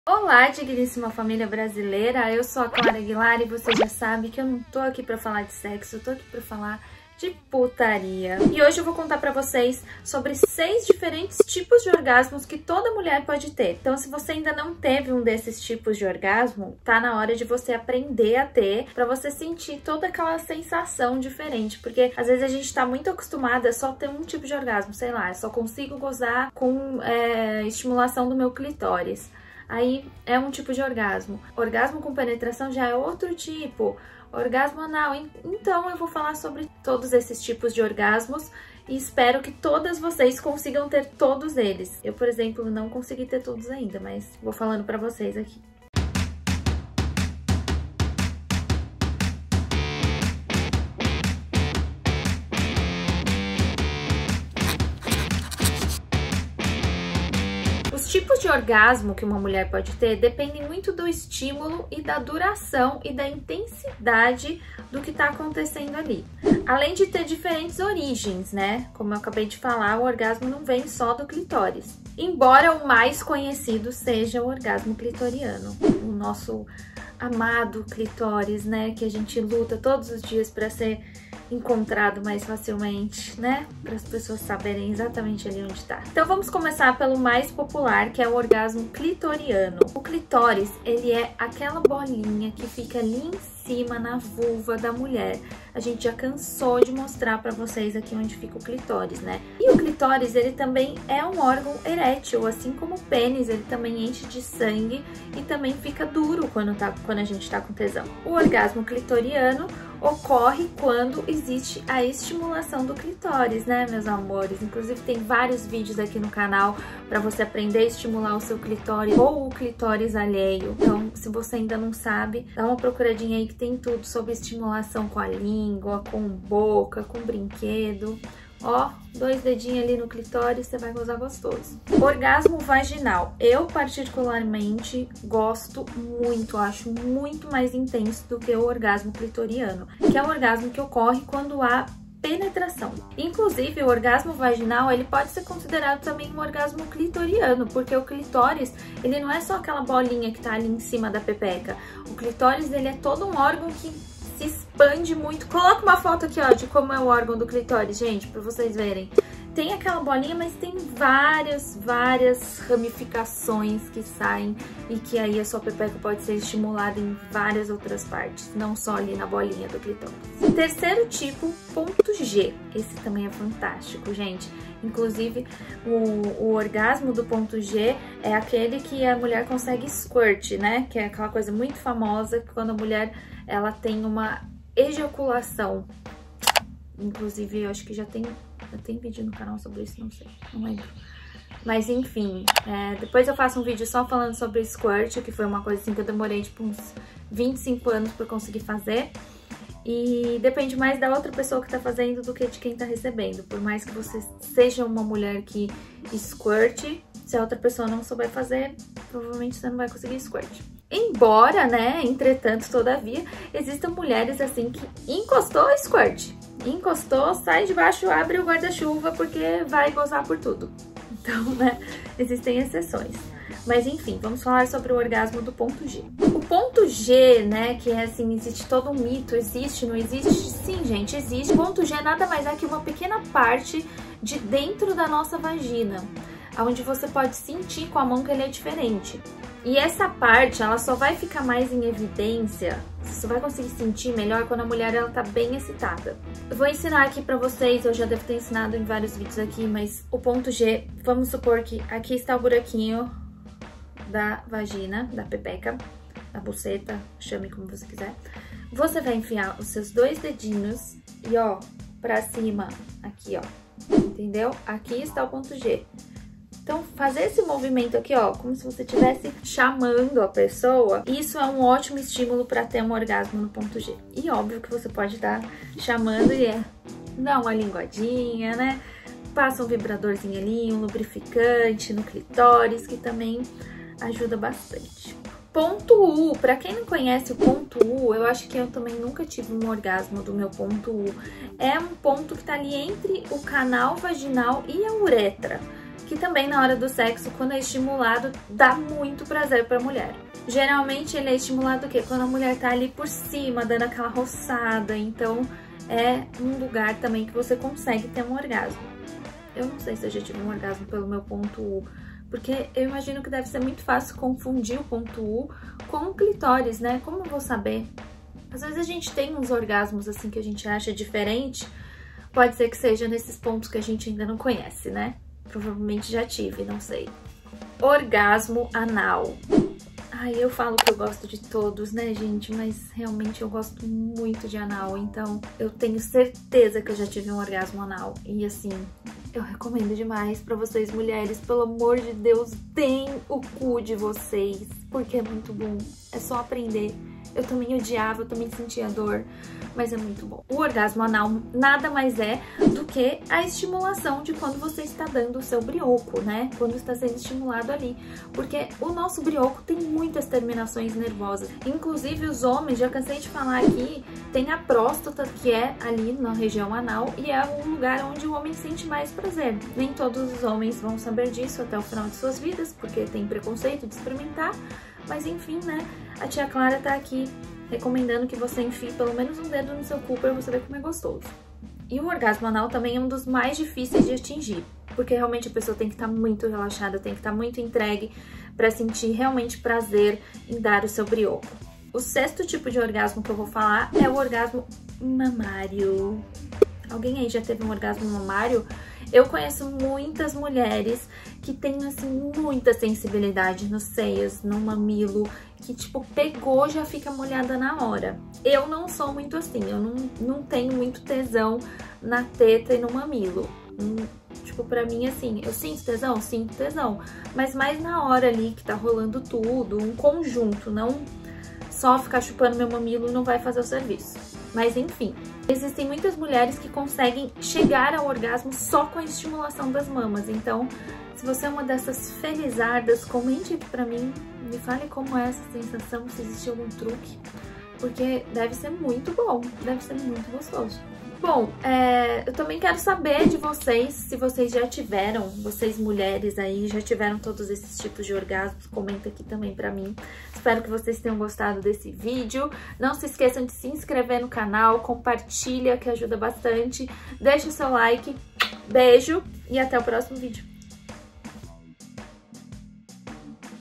TIPOS DE ORGASMOS FEMININO! | ÀS CLARAS
Hoje trouxe 6 tipos de orgasmos e quero que você comente ai pra mim, se já teve pelo menos um deles, compartilha esse vídeo com as coleguinha para todo mundo ter esse prazer na vida! ♥️